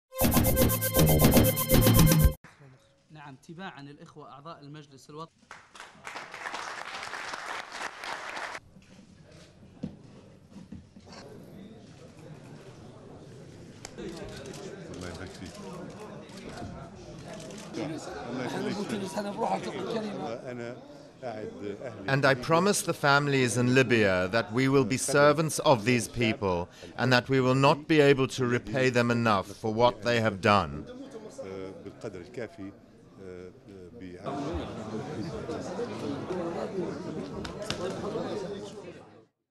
Libya's new PM faces huge tasks
Libya's interim rulers have elected technocrat and businessman Abdel Rahim al-Keib to head a transitional government as NATO ends the air campaign that plays a major role in ousting dictator Moamer Kadhafi. Duration: 00:39